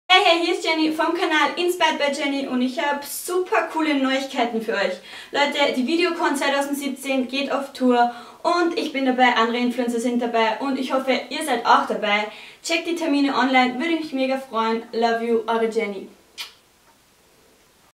inspiredbyDzeni kommt zur Influencer Video Con17 Dornbirn
Holt euch euer Autogramm und ein Selfie mit inspiredbyDzeni bei der InfluencerVideoCon17 in Dornbirn.